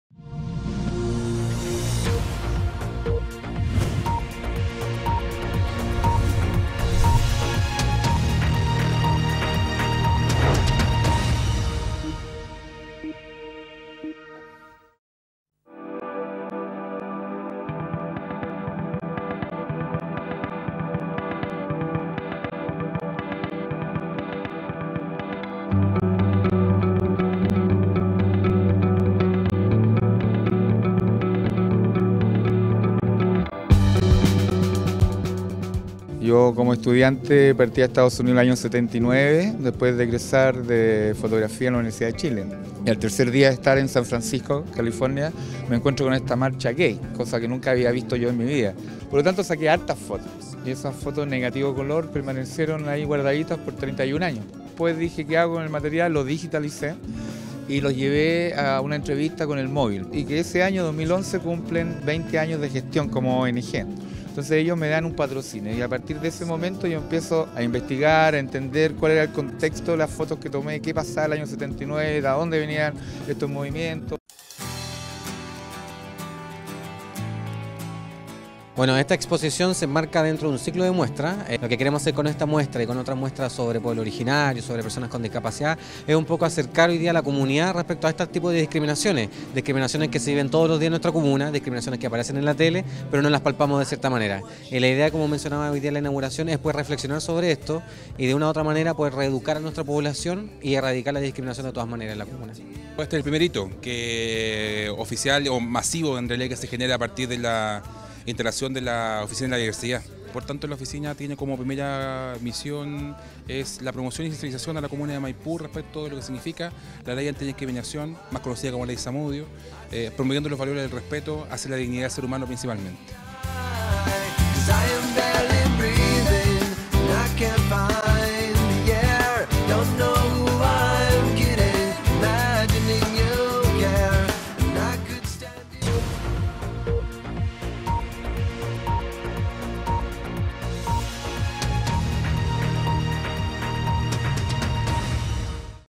SALIR DEL CLOSET
Con gran éxito se realizó la exposición fotográfica "Salir del Closet", en el Teatro Municipal de Maipú. La muestra presentó imágenes relativas a la lucha por los derechos de la diversidad sexual.